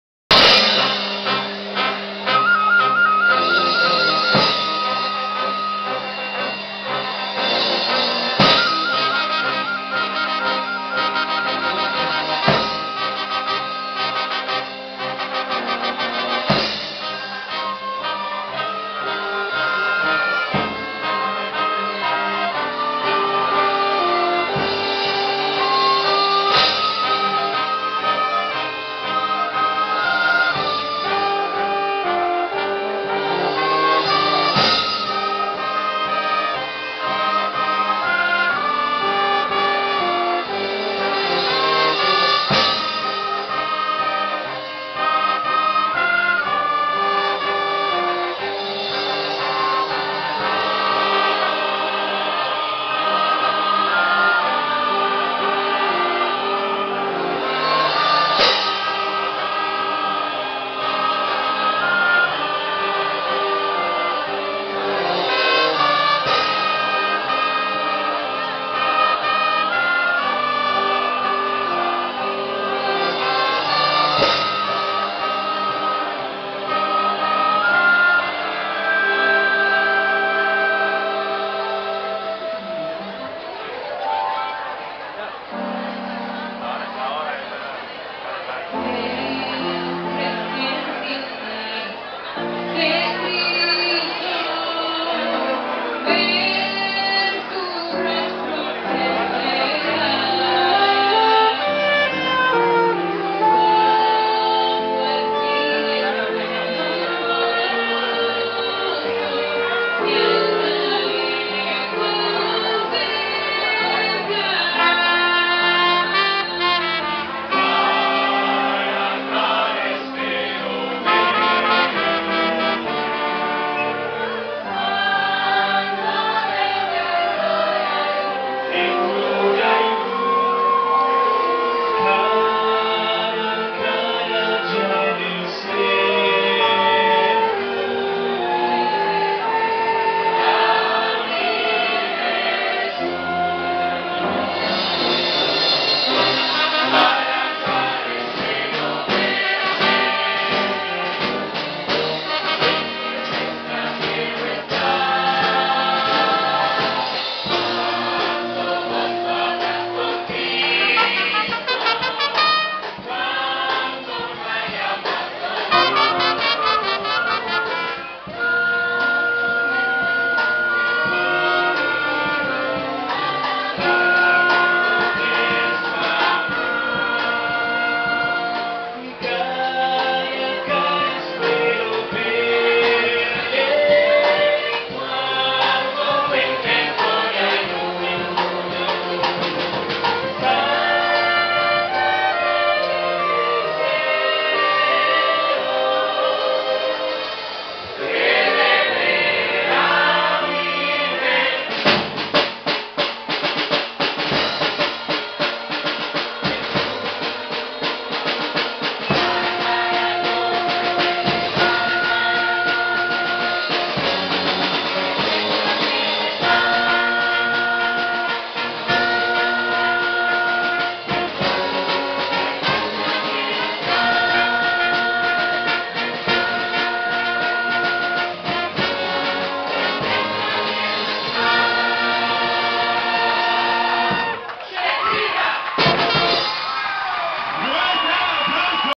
Talento de Guias Mayores SHEKINAH 2014
Este fue el Talento de la Escuela de Guías Mayores SHEKINAH de la Zona 1 de la Asociación Dominicana del Este, R.D, en el Camporee En SU Presencia 2014.